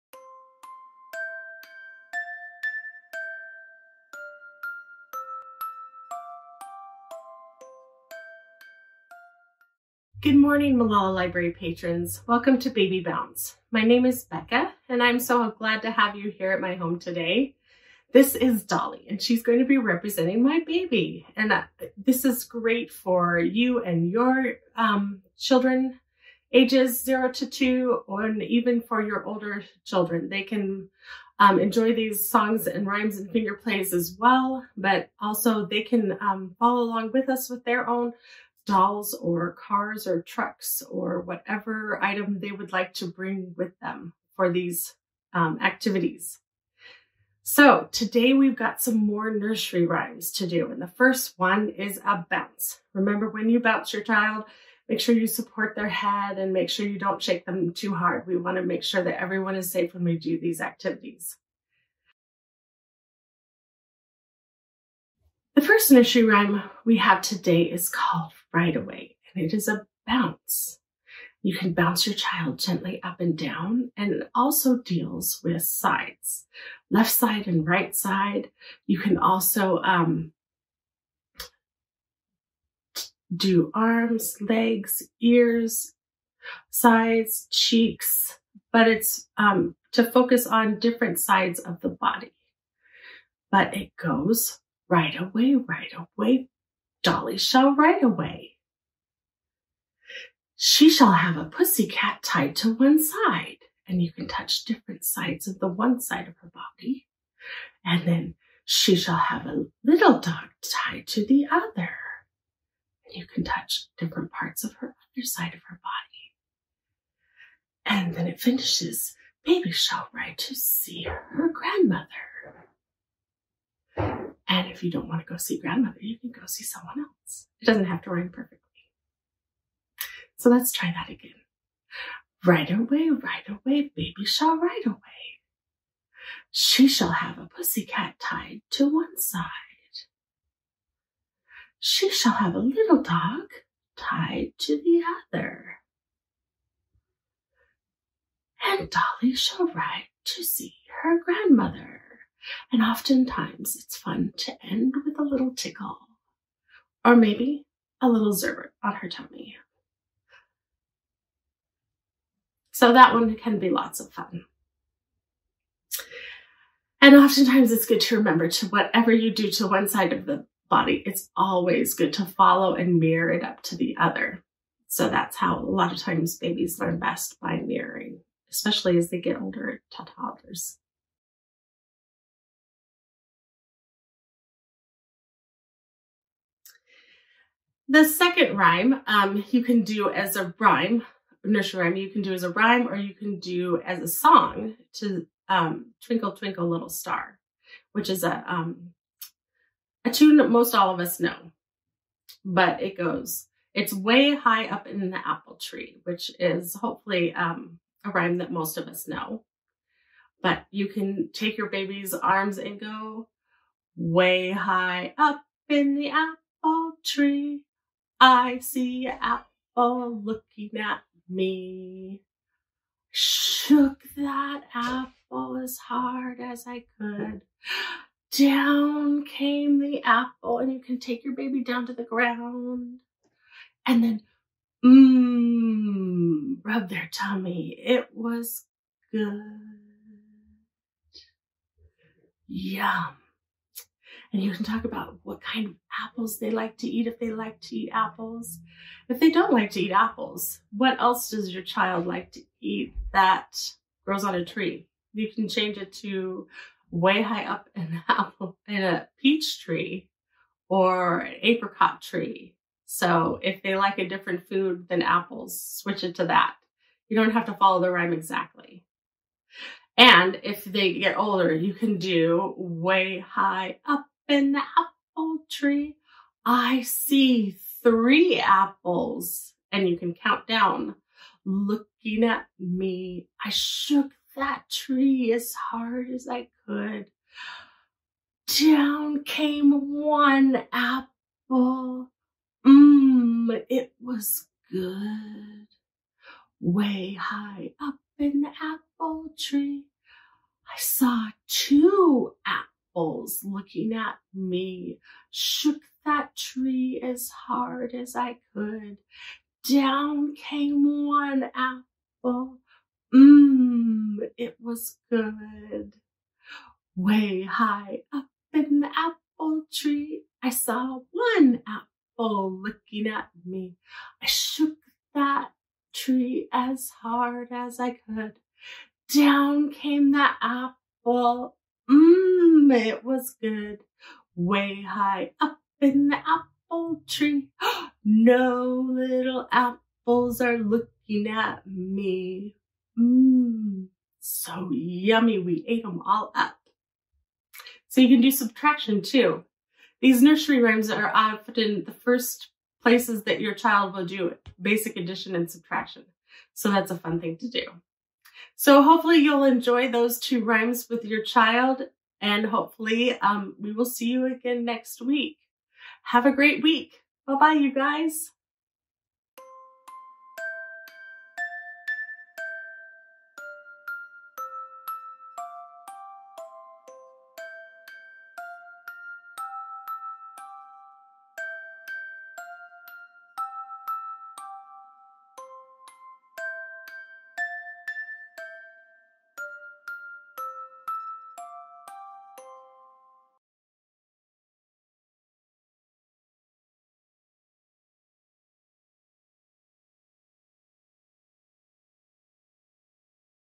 Nursery Rhymes, Part 3
Baby Bounce is designed for caregivers and their child, 0-2. Each week enjoy short interactions with Beka, Dolly, and your child as you learn new songs and rhymes to share with your child.